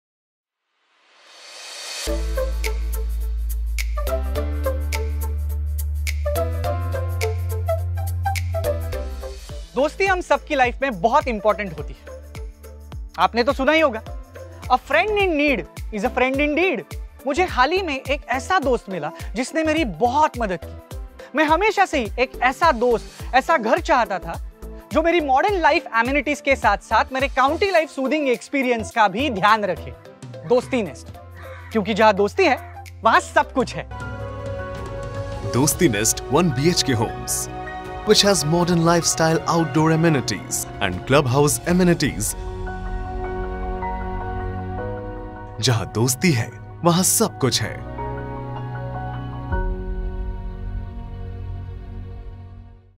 Modern, spacious, and beautifully designed - our new 1 BHK flats in Thane are waiting for you.
"Have you ever wondered what living in a modern day county life would be like? At Dosti Nest, Dosti West County, Thane (W), you'll get to experience the true essence of rural life with all its modern day amenities. Come explore this luxurious abode and see for yourself why it has become the perfect address for anyone looking for a peaceful and serene lifestyle. With its lavish amenities, beautiful landscaping and delightful entertainment options, Dosti Nest, Dosti West County promises an unmatched living experience. Watch this video to learn more about this one-of-a-kind residential paradise

Modern Day County Life at Dosti Nest, Dosti West County, Thane (W).

✅Located at Balkum in the heart of Thane
✅1 BHK (Smart, Spacia, Luxe) Homes
✅Expansive ground and podium garden with activity areas for all age groups
✅Dosti Club Nest with amenities catering to various preferences
✅Superior safety and security
✅ Environment-friendly design
✅770+ trees within Dosti West County - Dosti Nest (Sector 3)
✅Under construction Metro Line 5 Balkum station just 1 min away
✅Good social infrastructure like schools, shopping, hospitals, etc. in the vicinity